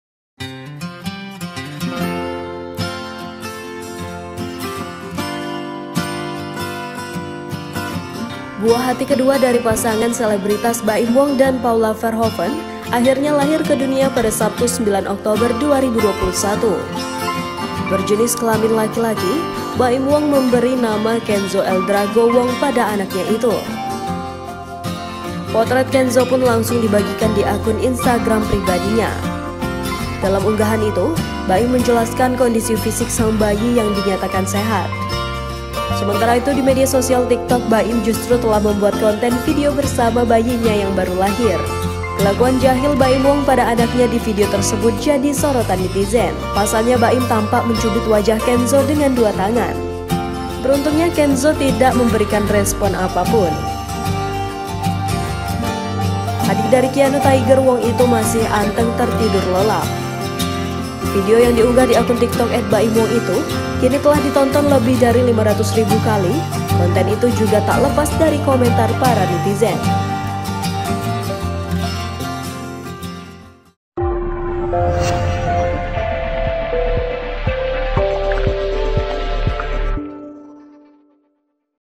NASIB ANAK KEDUA BAIM WONG, BARU LAHIR DIJAHILI DEMI KONTEN TIKTOK
TRIBUNJAMBI.COM-Buah hati kedua dari pasangan selebritas Baim Wong dan Paula Verhoeven akhirnya lahir ke dunia pada Sabtu (9/10/2021).

Berjenis kelamin laki-laki, Baim Wong memberinama Kenzo Eldrago Wong pada anaknya itu. Potret Kenzo pun langsung dibagikan di akun Instagram pribadinya.

Dalam unggahan itu, Baim menjelaskan kondisi fisik sang bayi yang dinyatakan sehat.